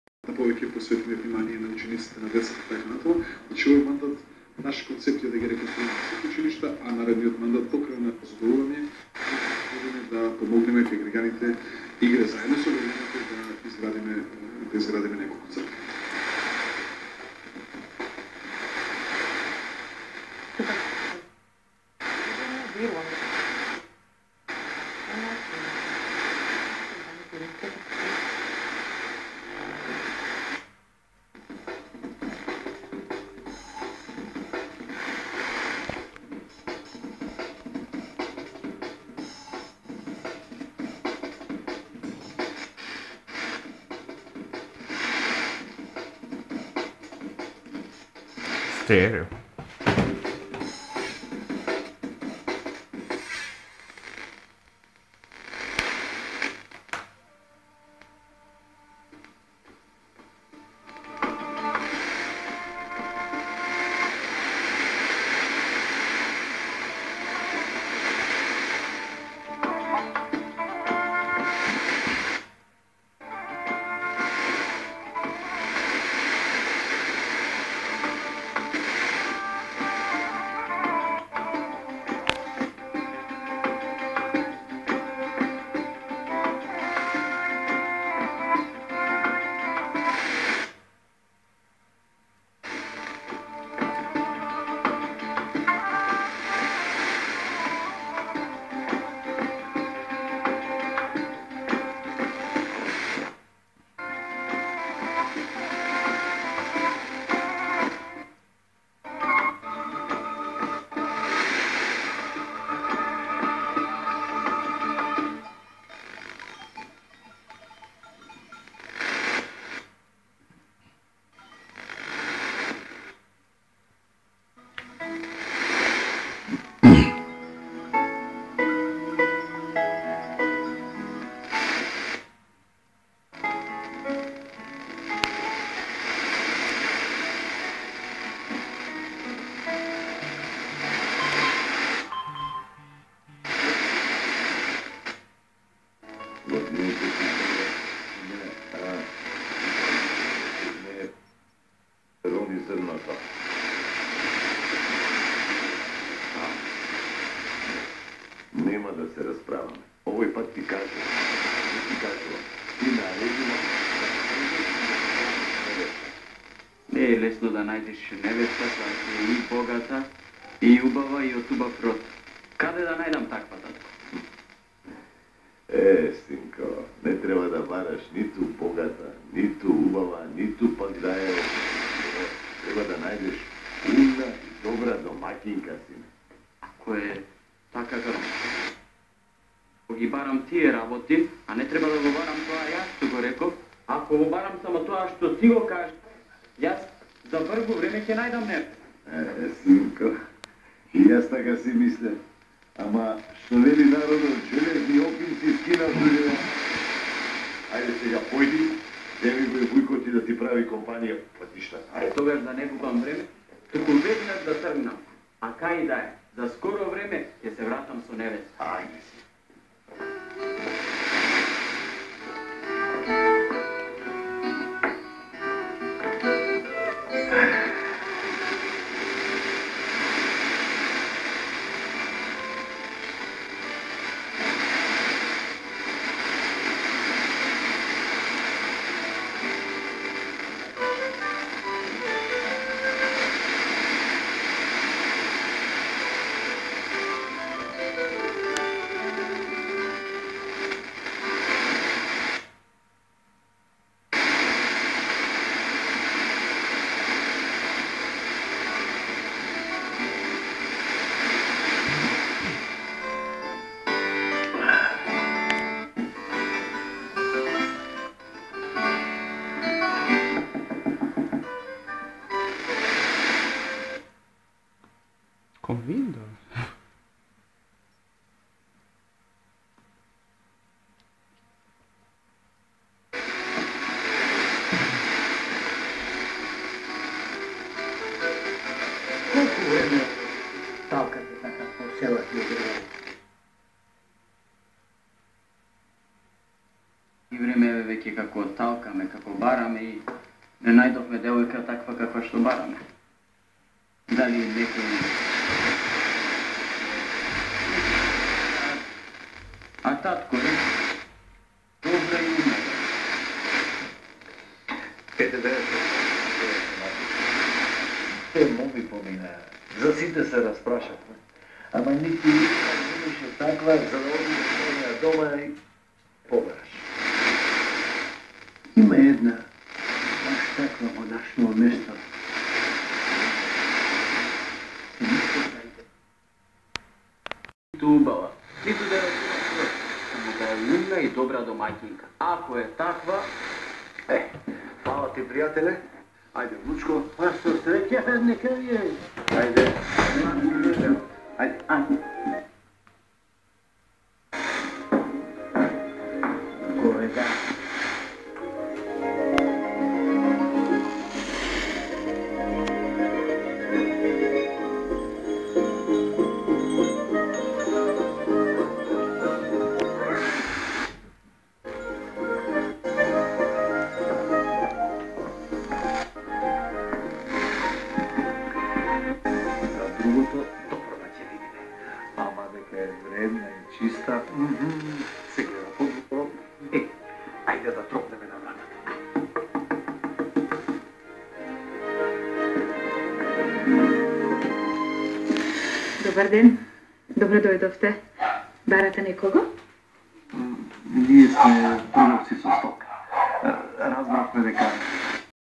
TV-DX Sporadic-E : FYROM MTB1 E4-62MHz in Biella,Piedmont [29-06-2012 h19]
TV-DX Sporadic-E : FYROM - MACEDONIAN MTB1 on E4-62,25MHz/PAL B (5,5MHz), Sometime received in STEREO; in province of Biella, Piedmont [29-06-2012 h19]

...that the second day for all this evening has received in color mode, and today Sat 30/Jun receiving another time that chan, third day in color! ...in first evening isn't received with your audio but an OIRT Radio with also folk music, unfortunately that file is corrupted :-/